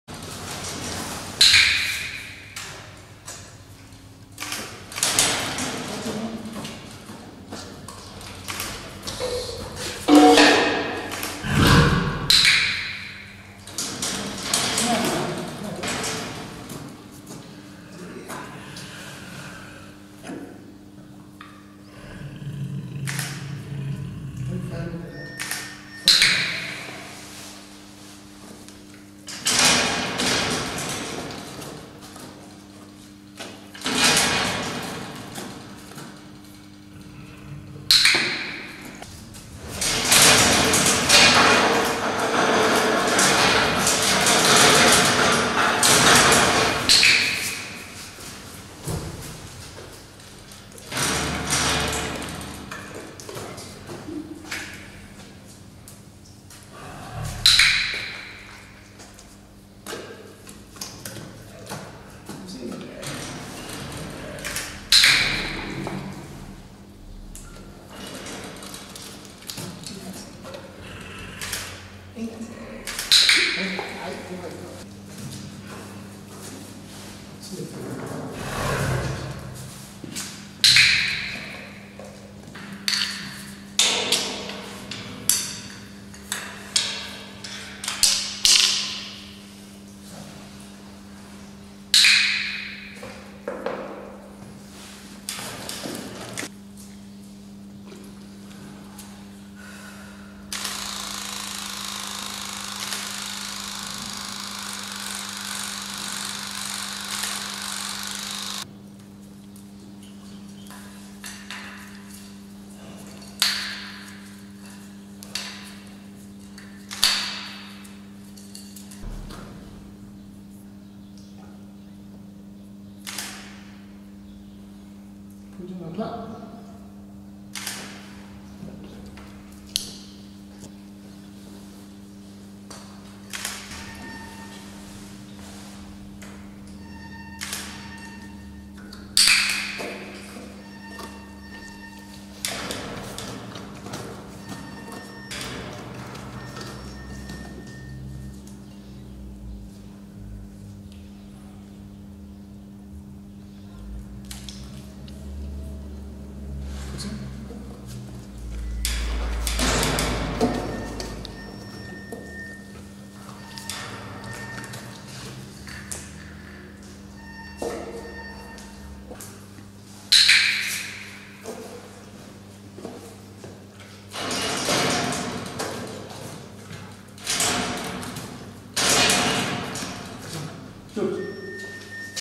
Tigertræning | Copenhagen Zoo
I denne video kan du se dyrepasseren træne hantigeren. Tigrene i Zoo trænes, så det er lettere for dyrepassere og dyrlæger at arbejde med dyrene og komme tæt på, når de skal undersøges. Her trænes tigeren bl.a. til at ligge stille, så der kan tages blodprøve og åbne munden så tænderne kan tjekkes.
In this video you will see the zookeeper in Copenhagen Zoo train the male tiger. The tigers are trained to make the daily work with the animals easier for the Zookeepers and so that veterinarians can get close. Here the tiger is trained to lie down and have a blood test done as well as open its mouth so the teeth can be checked.